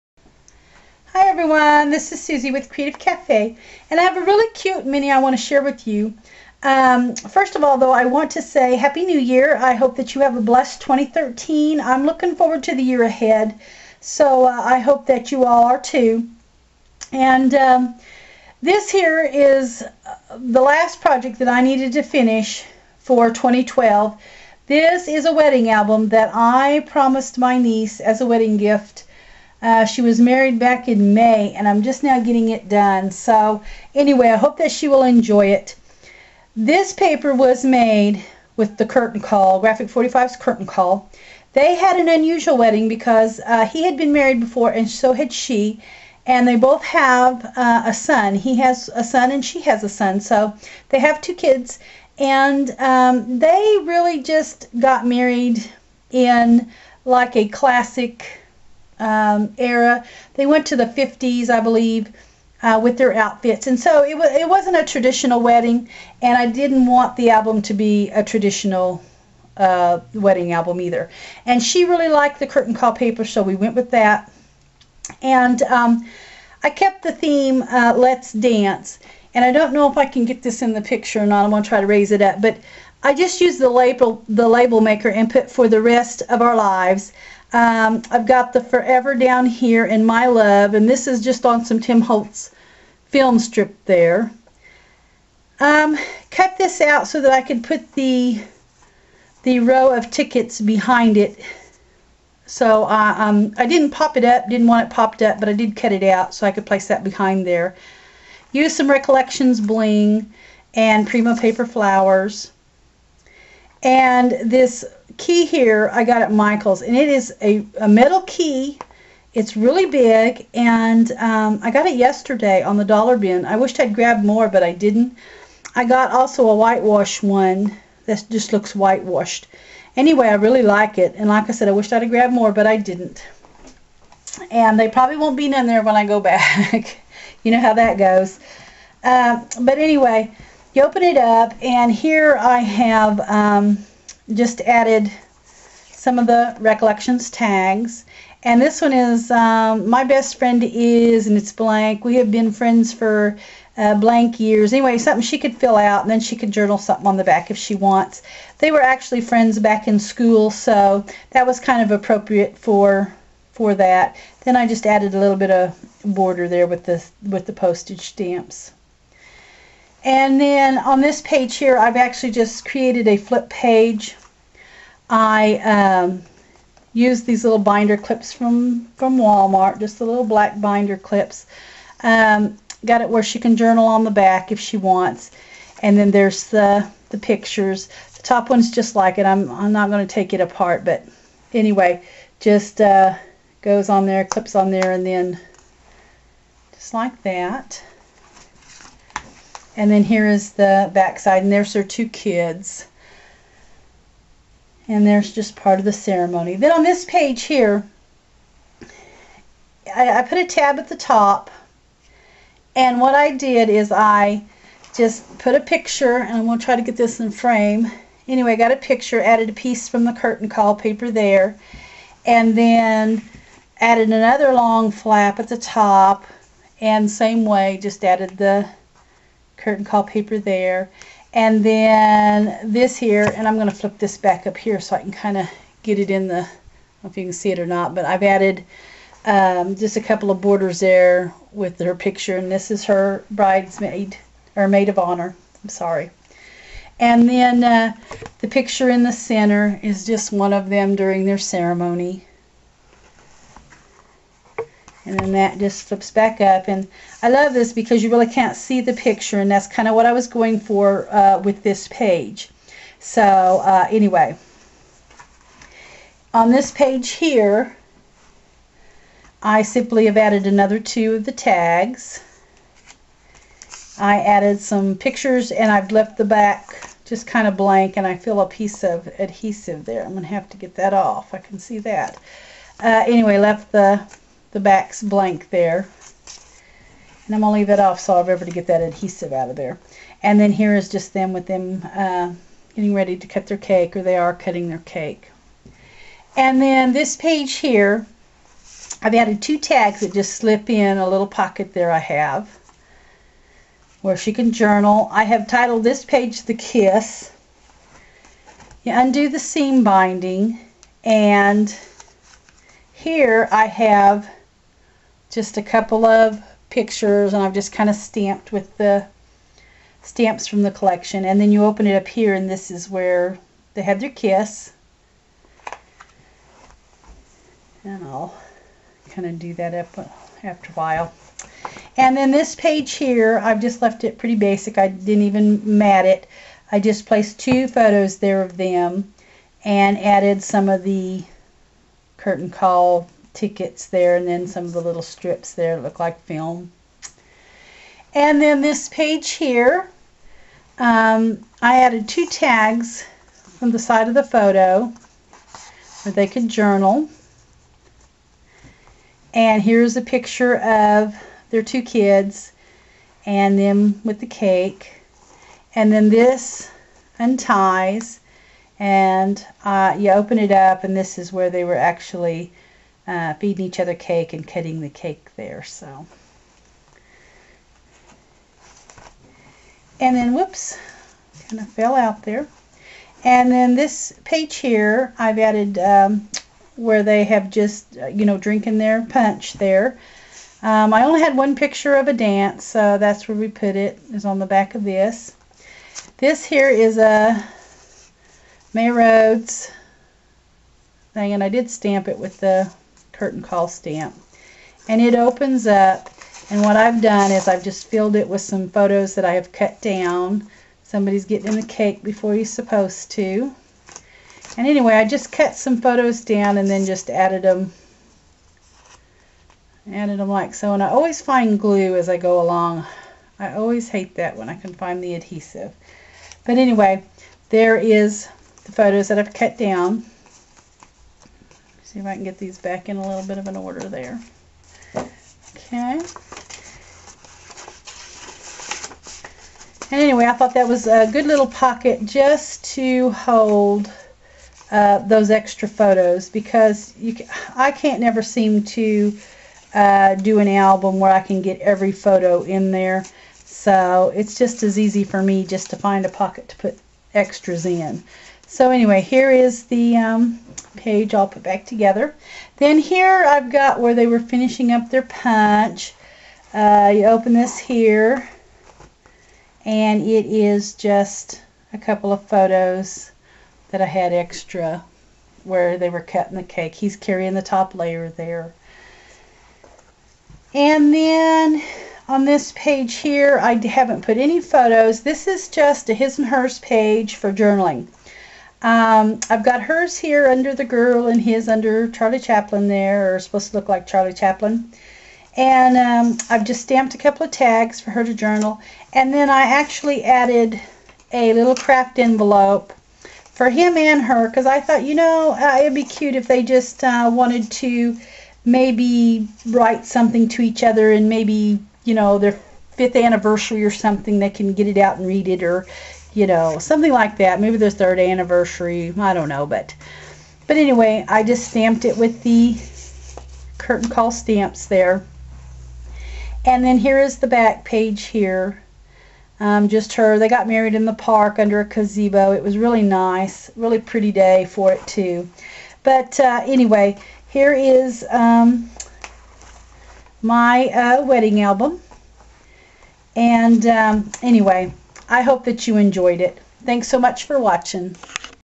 Curtain Call Wedding Album
I made this non-traditional wedding album for my niece using the Graphic 45 Curtain Call collection.
I invite you to stop by my blog from time to time
and my Scrapbook Junkies facebook page:http